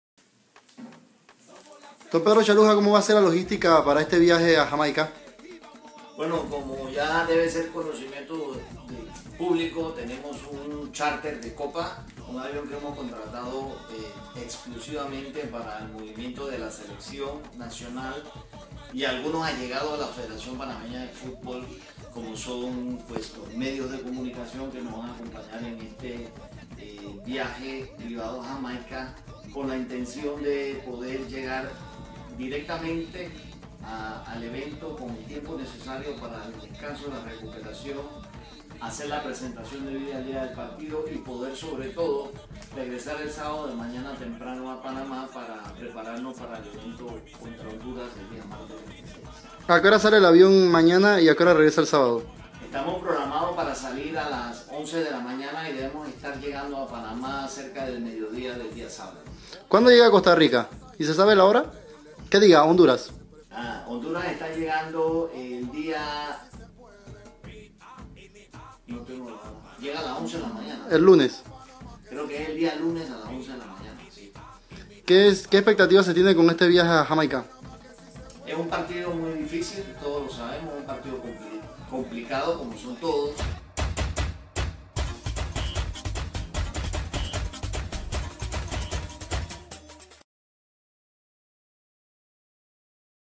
Pedro Chaluja Presidente FAPAFUT
El presidente de la FEPAFUT Pedro Chaluja adelanta los preparativos para el viaje hacia Jamaica y el partido contra Honduras